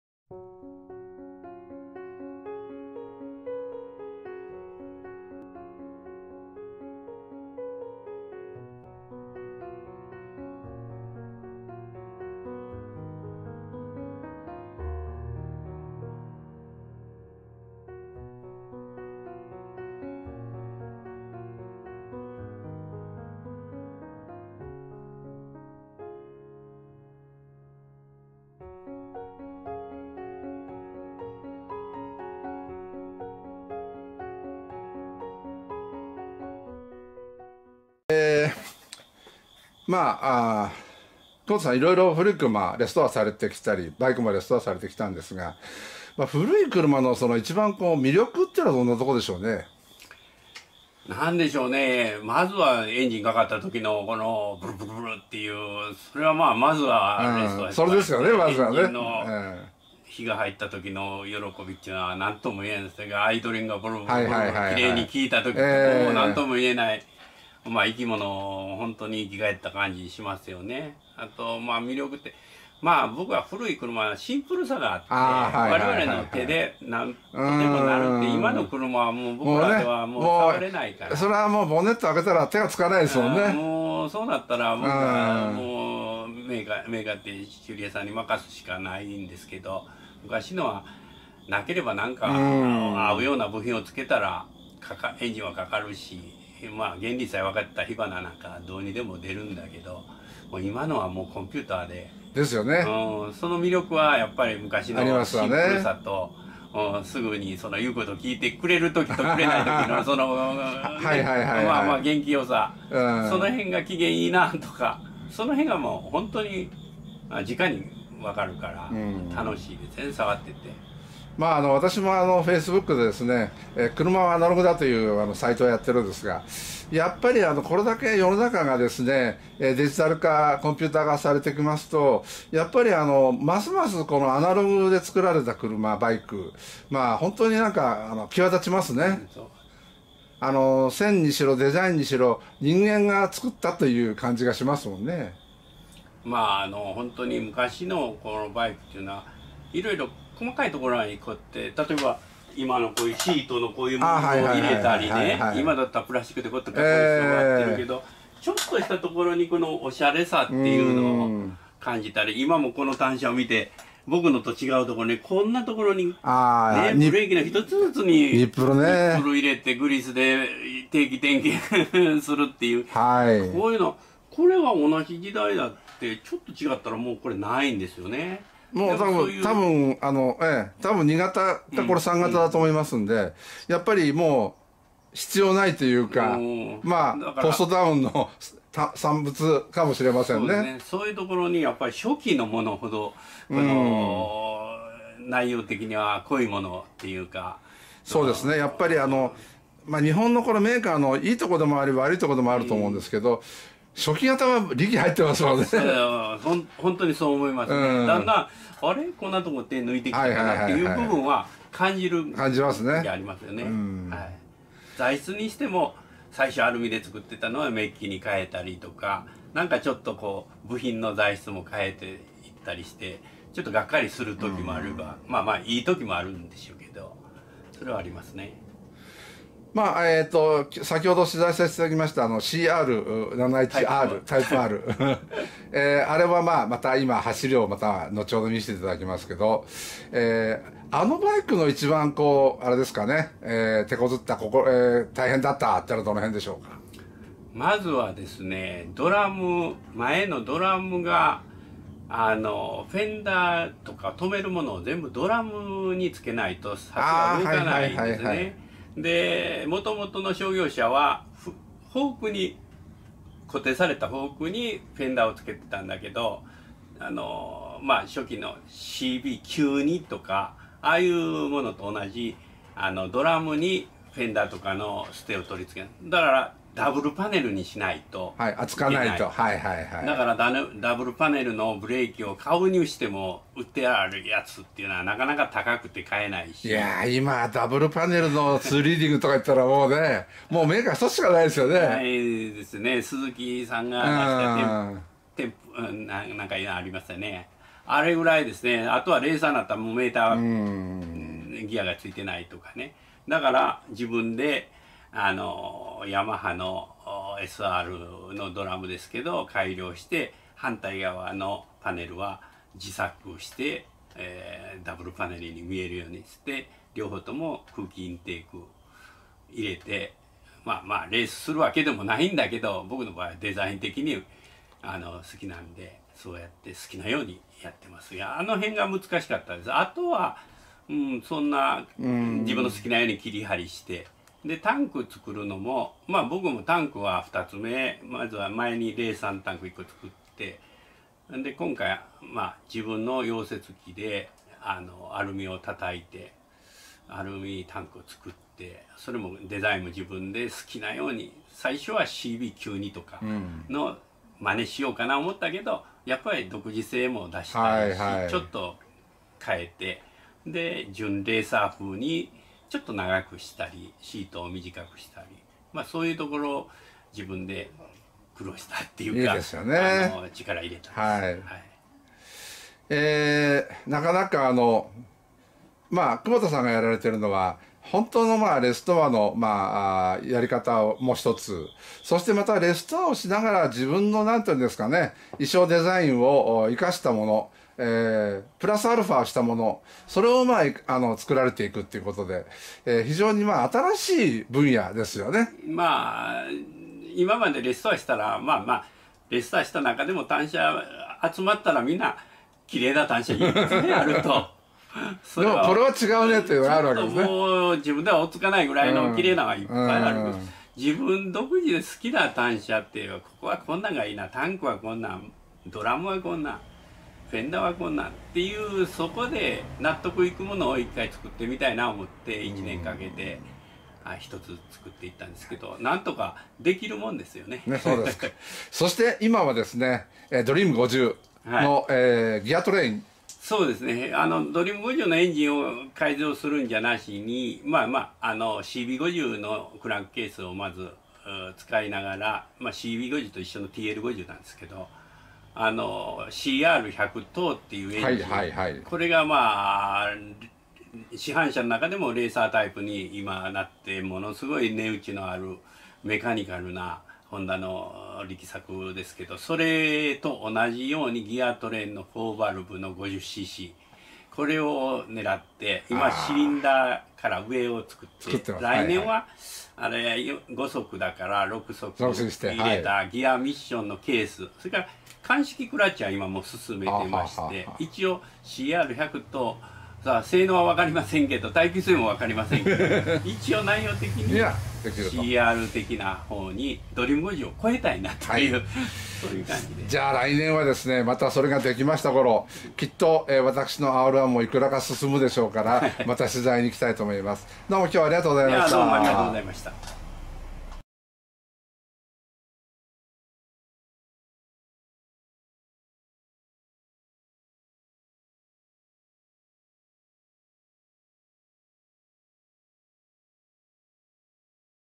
古都の匠 久保田技研 インタビューNO２
奈良で最高の技術と工夫で定評の「久保田技研」
の久保田所長のインタビューNO2です！！
ノスタルジックカーTV2＆４本体をご覧になる方は、こちらから